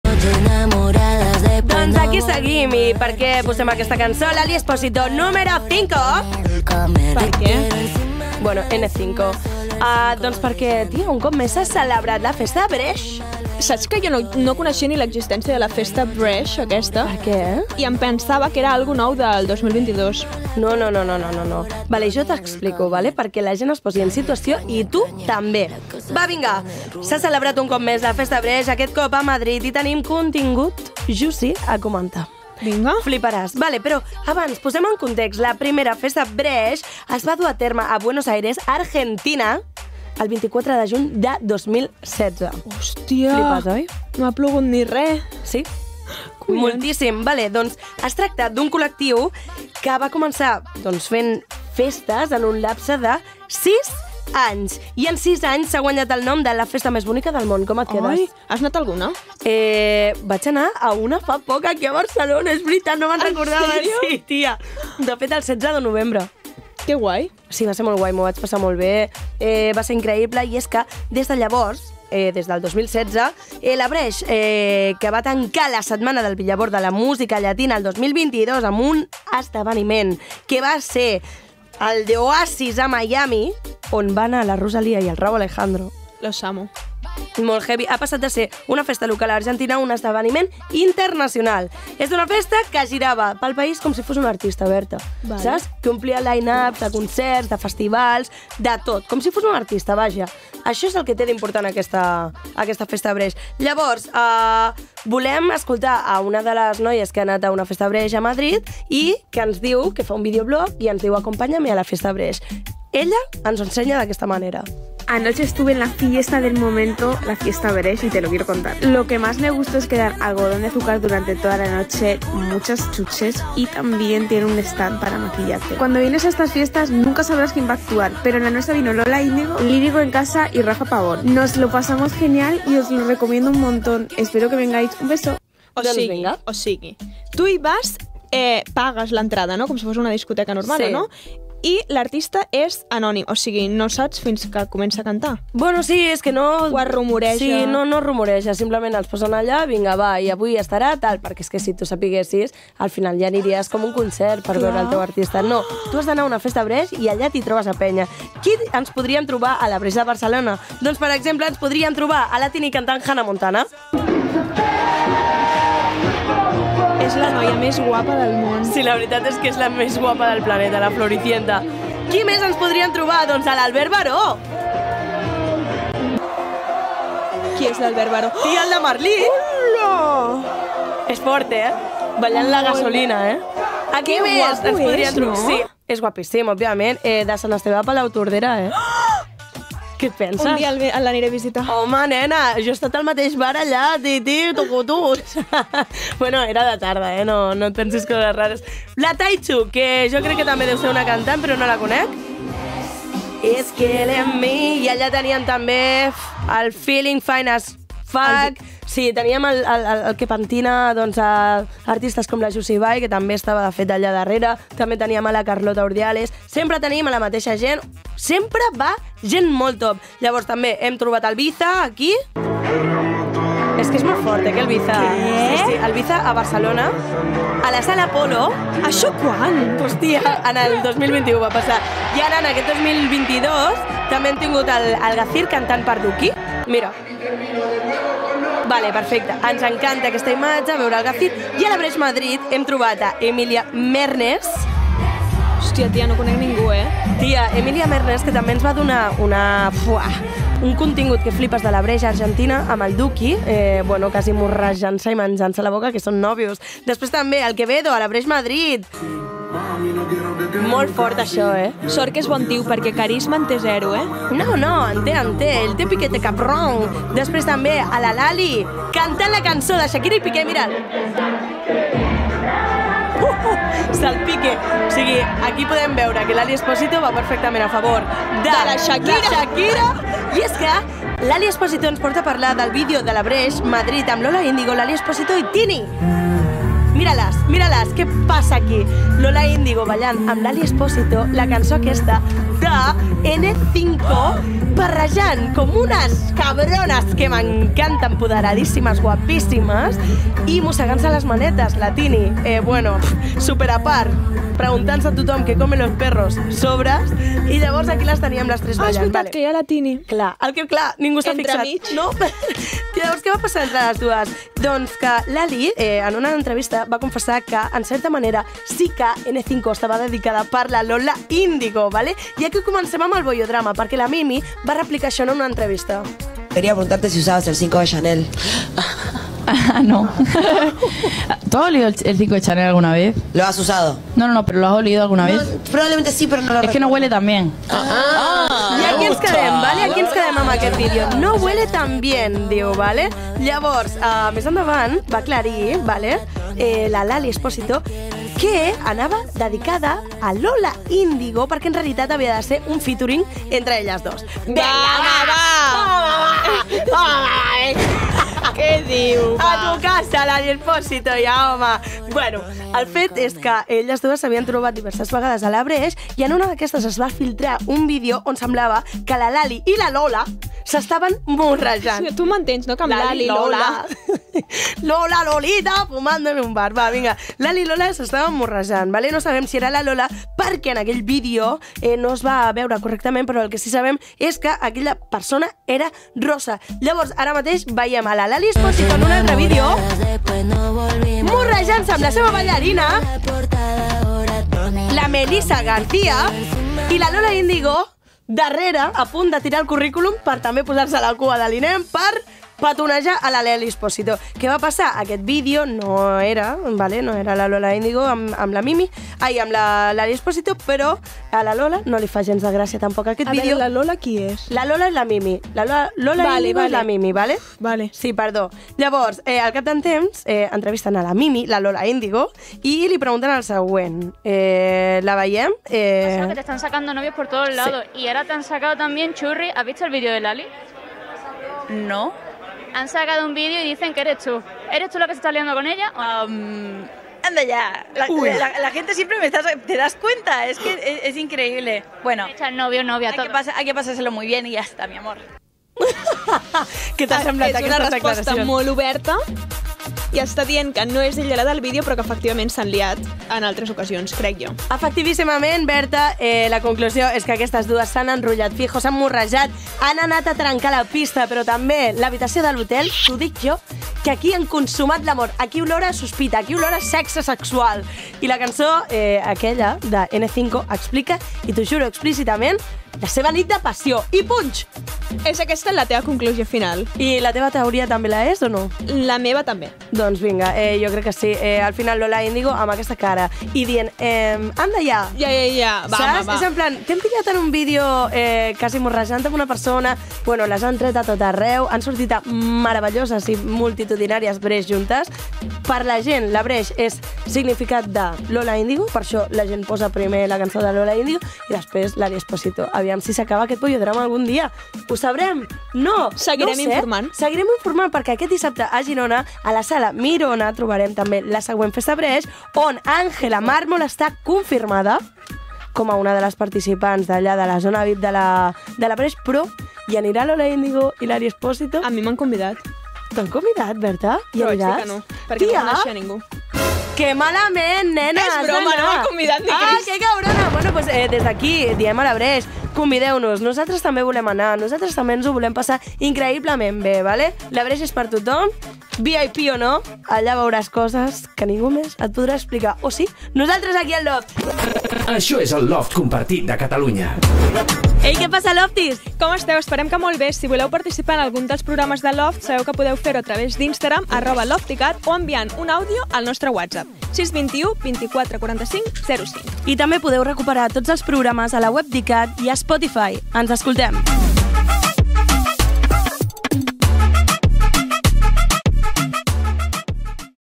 TOT EL QUE VA PASSAR a la FESTA BRESH
La Xènia Casado ens explica tot els safaretjos de la festes Bresh. Lola Índigo i Lali Expósito s'han embolicat. Ups, us he estripat el final.